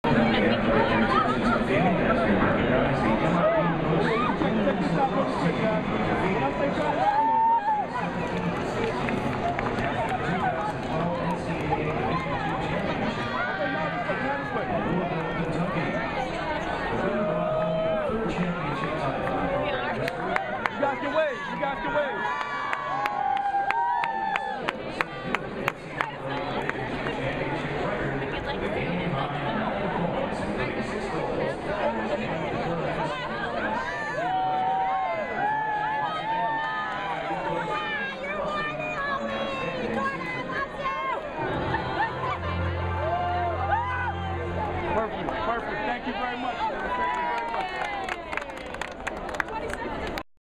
LIU Post Women's Lacrosse Mets Ceremony 7/23/12
The LIU Post women's lacrosse team was honored by the New York Mets in a pregame ceremony on July 23 for winning their third NCAA national championship title.